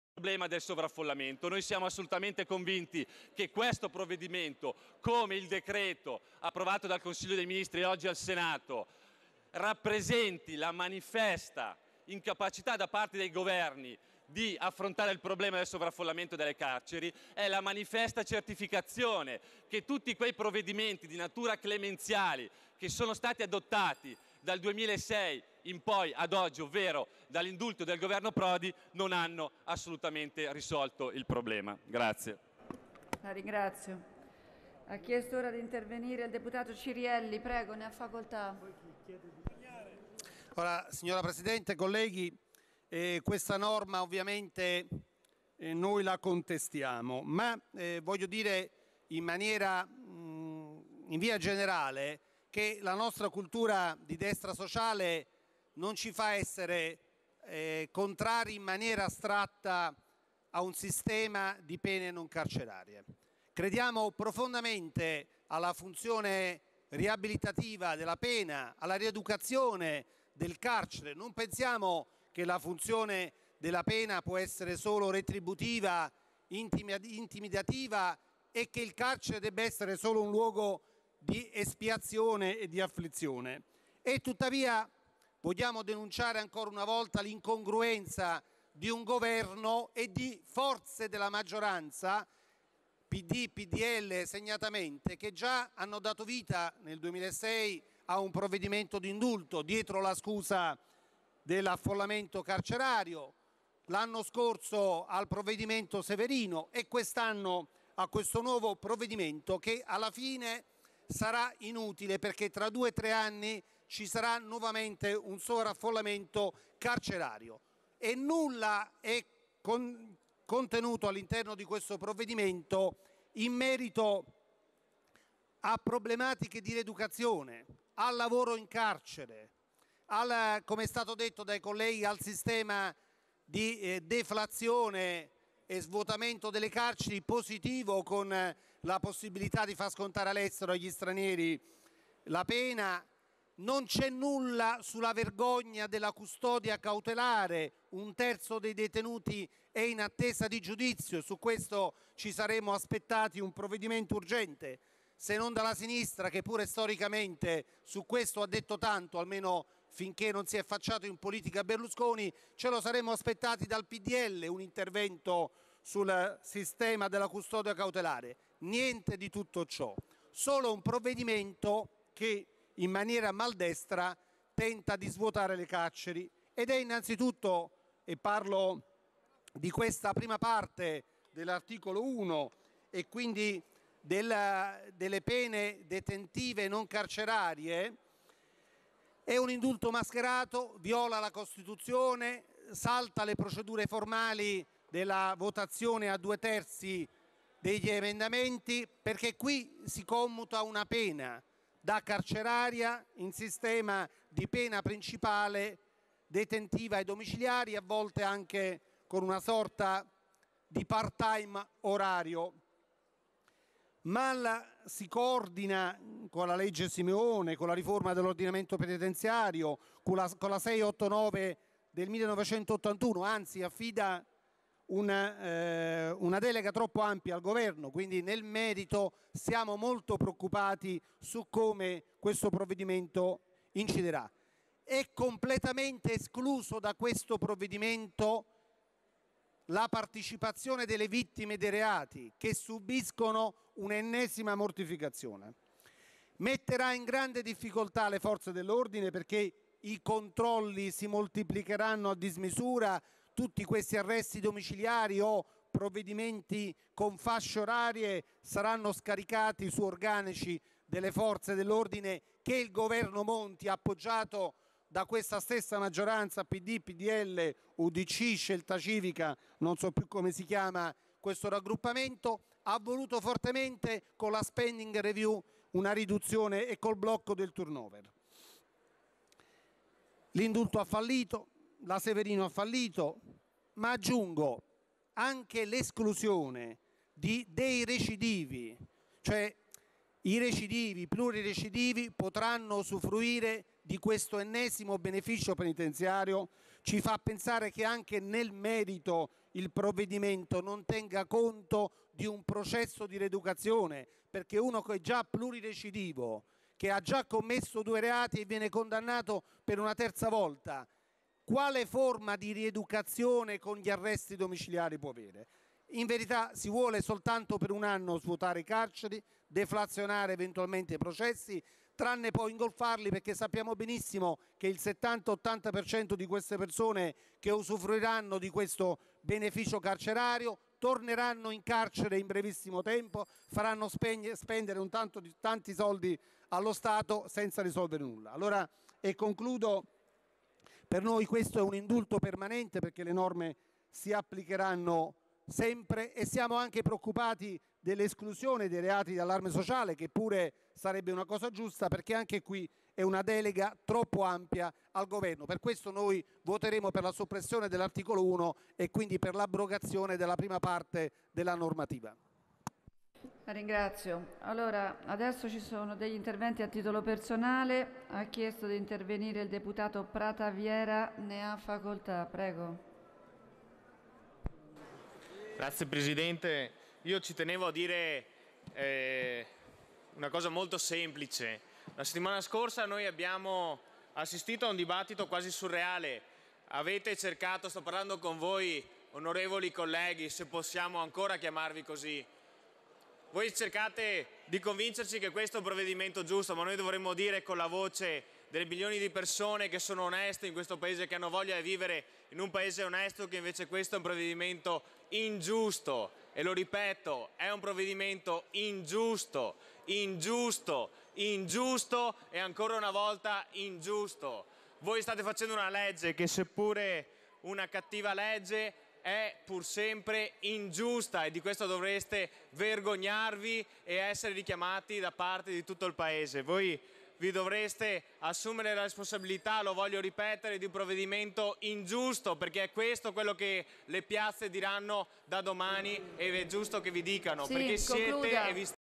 Intervento CIRIELLI EDMONDO - 2013 07 02 15 37 59 - Messa alla prova
Intervento on. Edmondo Cirielli - La nostra cultura di destra sociale non ci fa essere contrari in maniera astratta ad un sistema di pene non carcerarie. Crediamo profondamente nella funzione riabilitativa della pena e nella rieducazione in carcere, ma denunciamo l'incongruenza del Governo e delle forze di maggioranza, Pd e Pdl segnatamente, che già nel 2006 hanno dato vita ad un provvedimento di indulto dietro la scusa dell'affollamento carcerario, l'anno scorso al provvedimento Severino e quest'anno a quest'ultimo provvedimento che si rivelerà inutile, perché tra due-tre anni il problema del sovraffollamento si ripresenterà.
All'interno del provvedimento non c'è nulla sul problema della rieducazione, sul lavoro in carcere, sul sistema della deflazione, sulla possibilità di far scontare il carcere agli stranieri nei loro Paesi d'origine. Non c'è nulla sulla vergogna della custodia cautelare. Un terzo dei detenuti è in attesa di giudizio. Su questo ci saremmo aspettati un provvedimento urgente, se non dalla sinistra che storicamente su questo pure ha detto tanto almeno finché non si è affacciato in politica Berlusconi, dal Pdl.

Delega al Governo in materia di pene detentive non carcerarie e disposizioni in materia di sospensione del procedimento con messa alla prova e nei confronti degli irreperibili.(C. 331-927-A) (FAS) - 2 luglio 2013